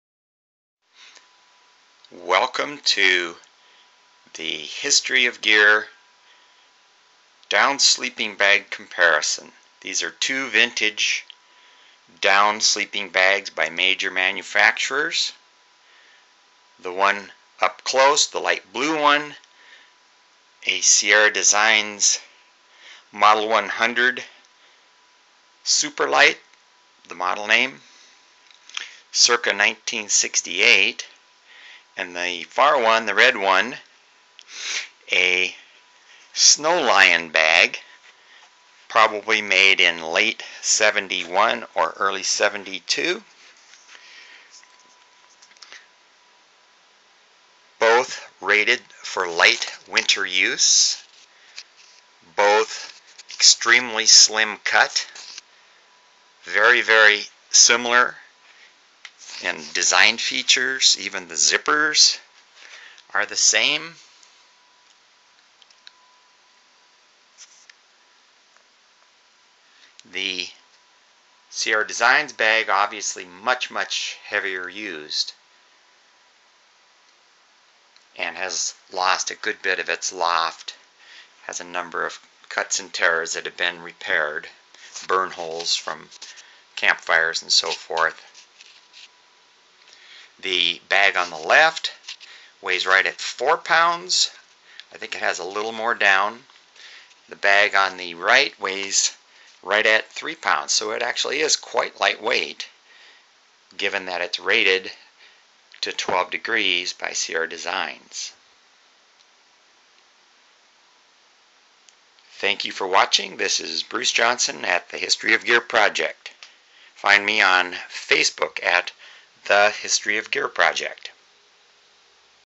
2 vintage down bags compared
Comparing two vintage down sleeping bags of the slim-cut type, aka "mummy bags." Sierra Designs 1968 vs. Snowlion 1971.